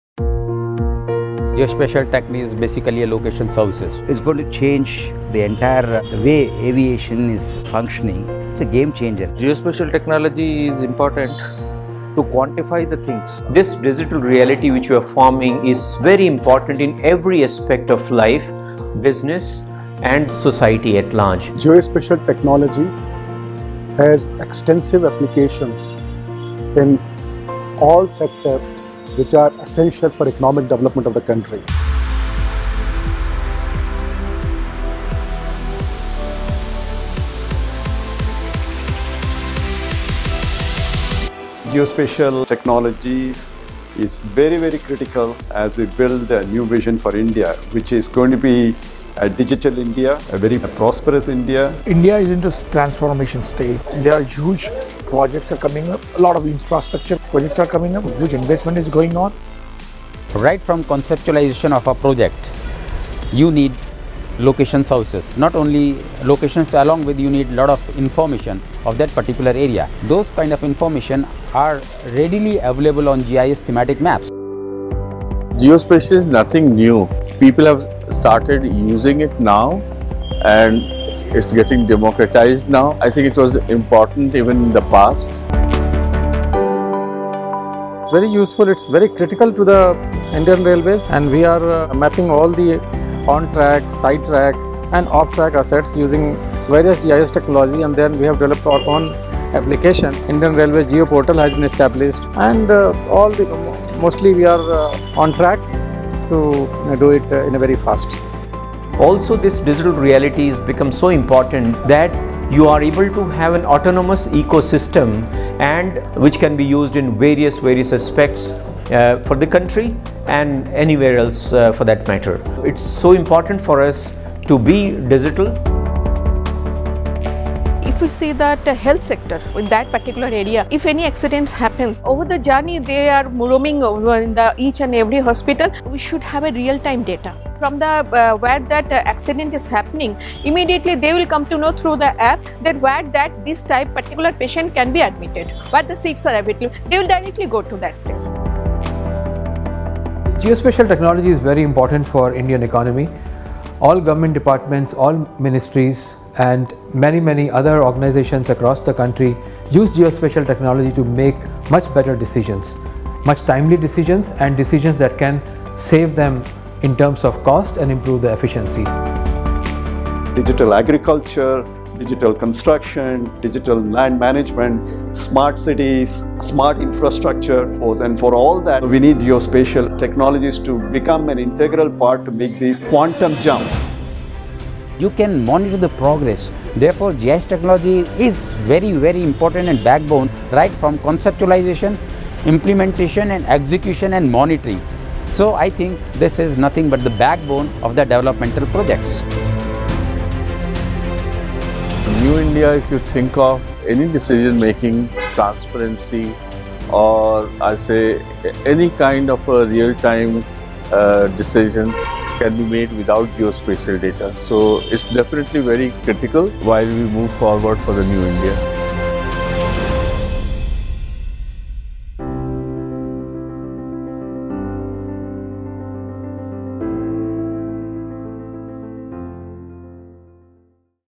Vision New India: Digital Transformation through Geospatial Innovation
India is on the anvil of a major evolution, which is largely supported by technical advancements that will give rise to a digital world where efficient and smart systems will enable in building a new, clean, healthy, secure, inclusive and robust India.

In order to advance on its vision, the Government has laid out plans for Digital India, Smart Cities, High Speed Railways, Irrigation and Skill Development, aiming to create a sustainable base for continuing this development into the next decade. Geospatial technology is essential to each of these programs as it provides a platform that is capable of assimilating real-time data, provides an efficient processing system and the delivers a shared, visual and lucid product that facilitates effective, efficient and informed decision making. The integration of geospatial technology in the New India Vision will enable the development of applications that have the potential to revolutionize productivity and efficiency across the industry.